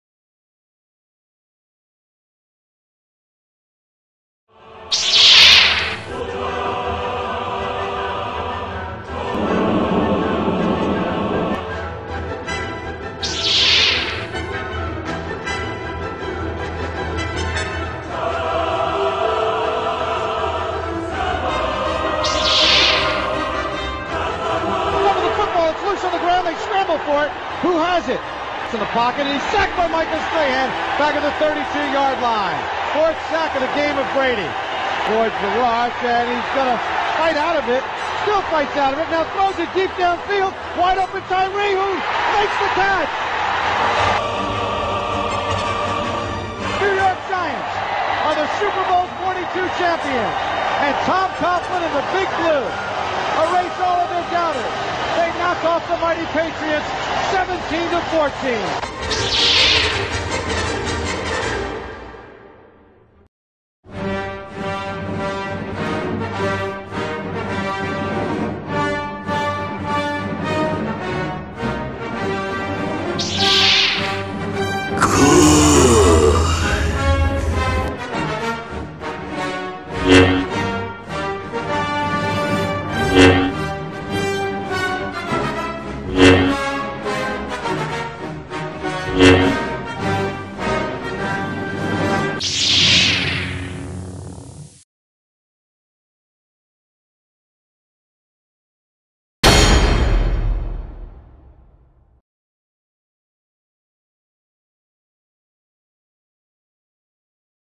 Super Bowl 46 Trailer
Super Bowl 46 Movie Trailer. In this most anticipated sequel to Super Bowl 42, everyone wants to know... will the Empire Strike Back?